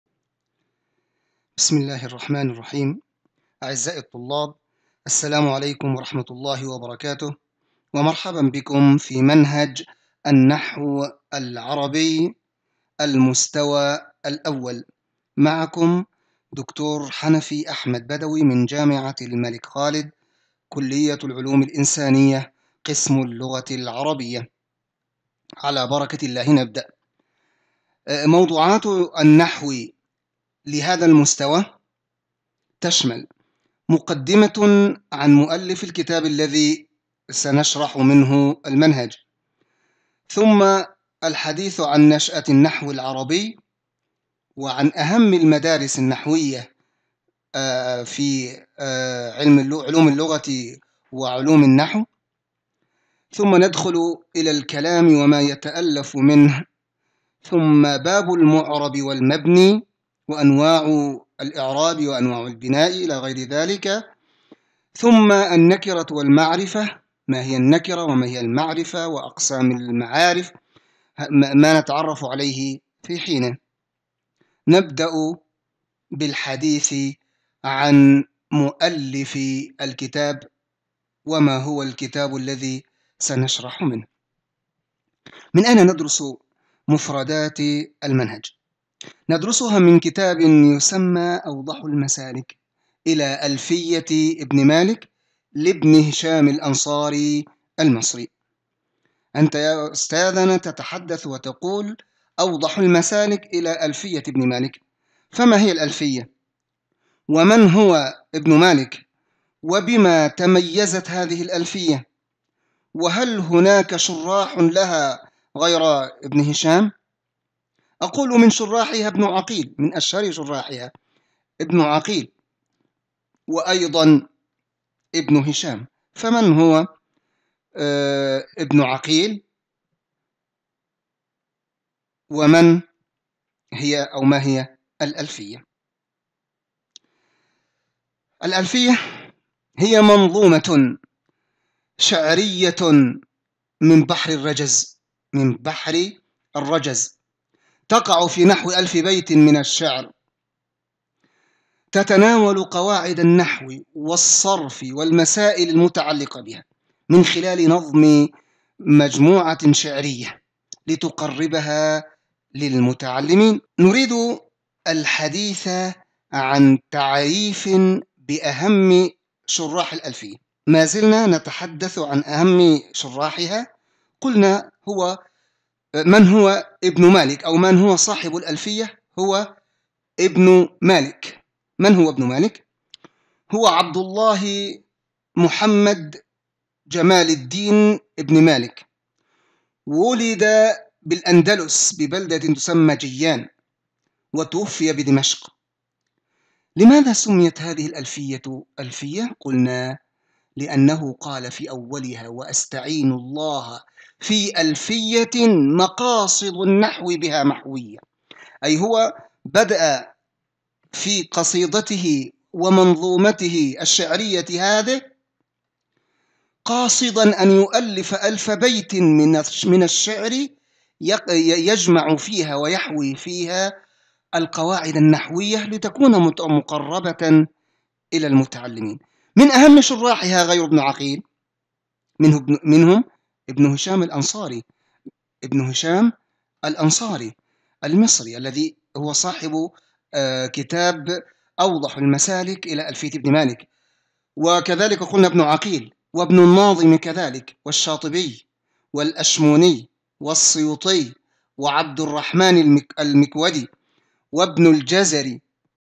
النحو العربي | 1-1 | مقدمة عامة لمقرر النحو العربي
الوحدة 1 - الدرس 1 : مقدمة عامة لمقرر النحو العربي